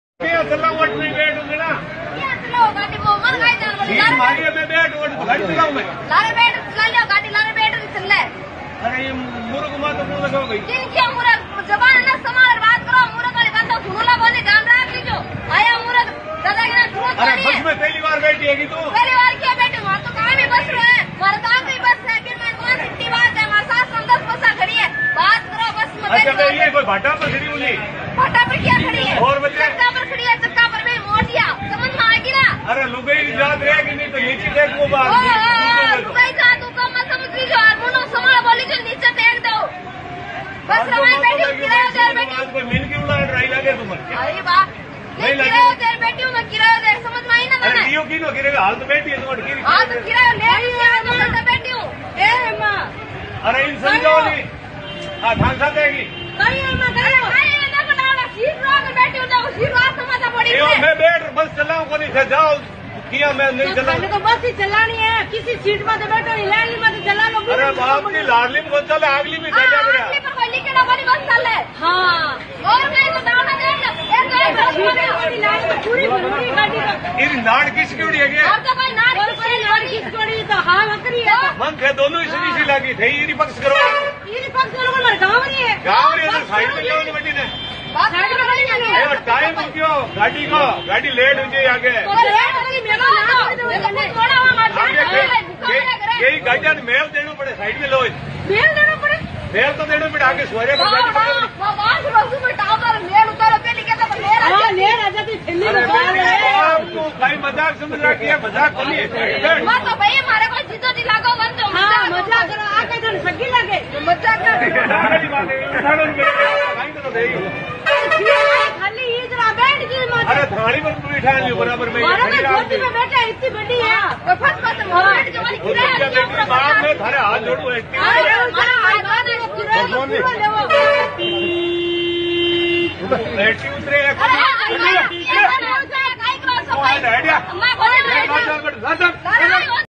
Indian lady fights with bus driver
It can only happen in INDIA

A lady and her bahu get into a crowded bus in Rajasthan. The mother gets a seat. The bahu too searches for one, sees the driver's seat vacant and occupies it. When the driver comes and asks the lady to vacate his seat, she tells him to go behind and sit wherever. When he says, he has to drive the bus, she asks him to drive from any other seat. Her mother in law too supports her.. Real incident captured